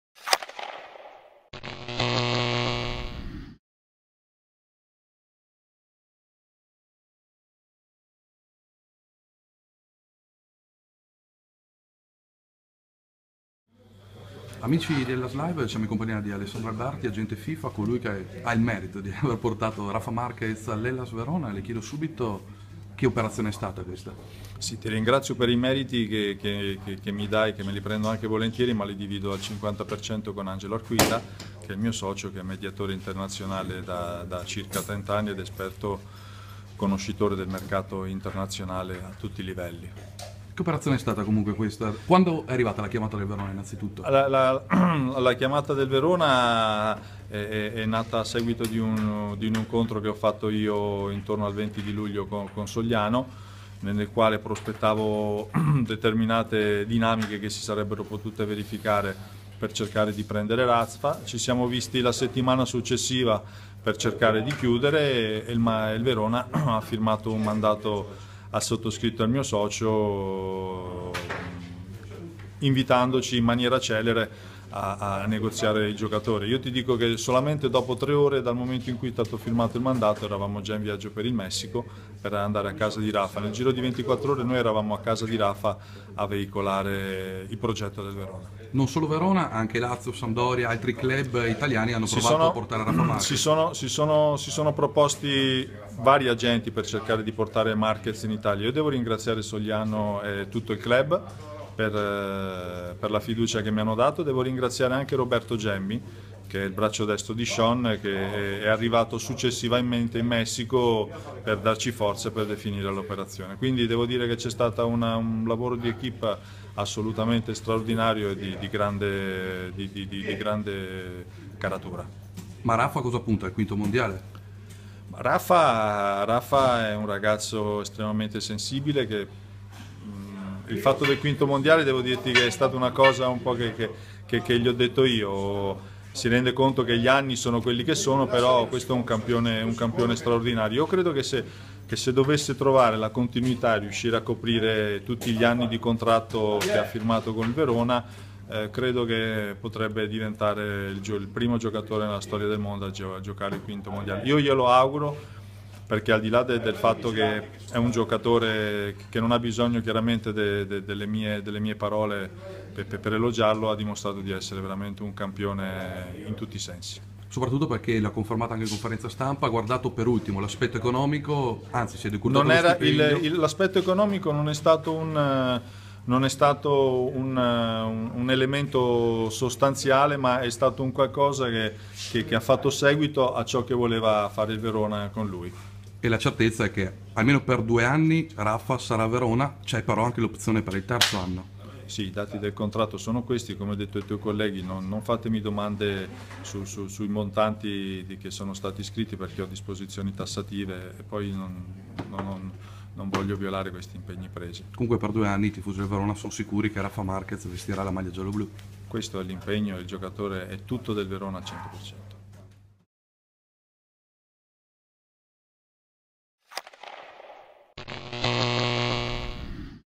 L'affare Marquez
L'agente FIFA Alessandro Alberti spiega a HELLAS LIVE i termini e le modalità con cui l'Hellas Verona è riuscito a portare in gialloblù il difensore messicano